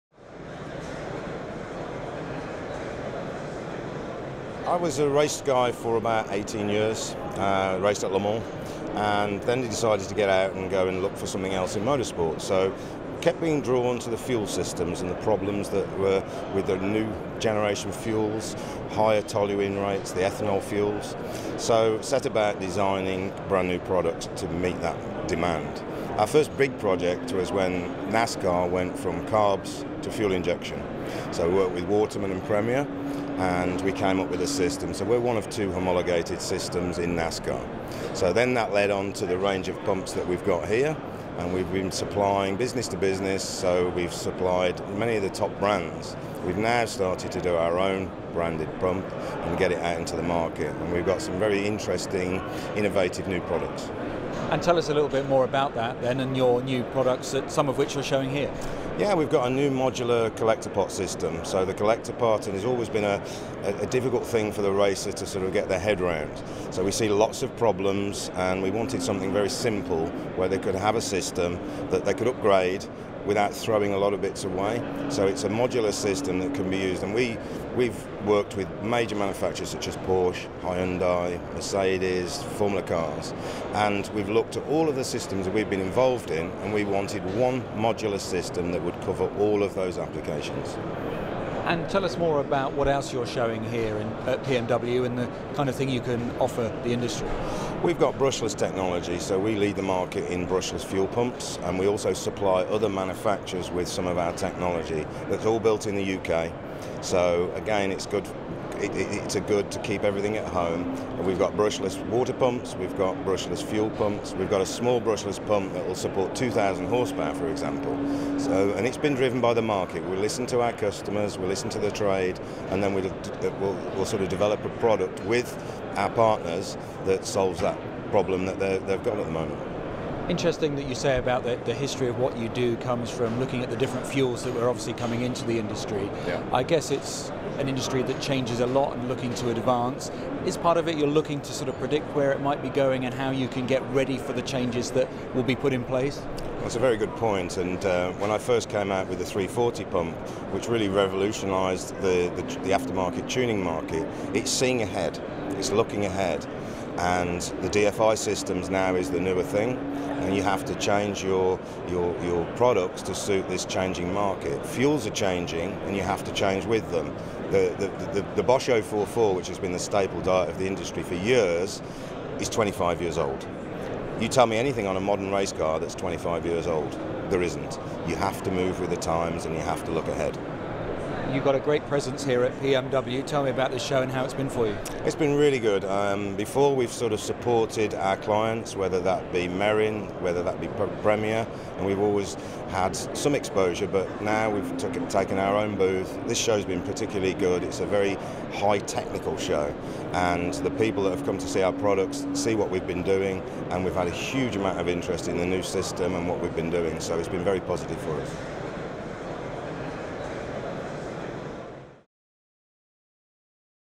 Professional MotorSport World Expo 2016 exhibitor interview – Protec Fuel Systems
Protec Fuel Systems, UK, is a specialist in the design and manufacture of fuel pumps and complete systems for motorsport, low-volume OE applications with specific flow and pressure requirements and brushless fuel pumps. Rob Schirle, head of design & engineering, showcases the products on the company’s stand.